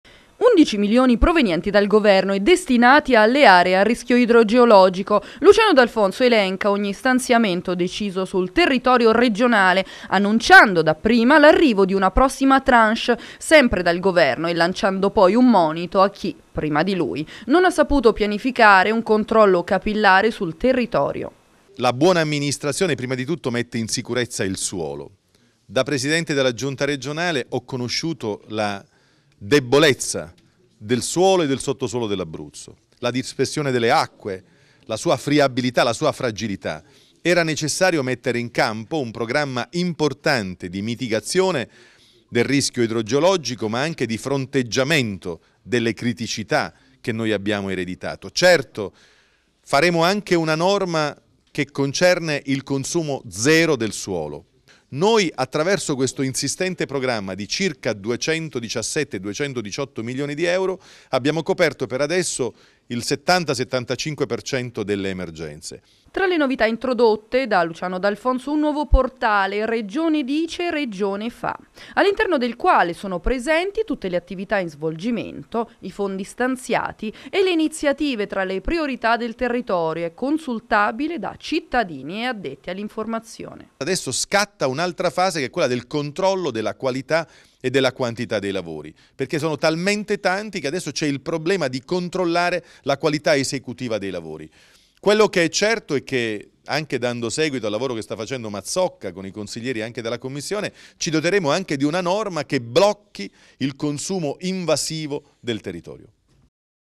D'Alfonso. 10 milioni per il rischio idrogeologico
Il Presidente della regione questa mattina ha annunciato l'arrivo di più di 10 milioni di euro, provenienti dal governo, destinati alle aree a rischio idrogeologico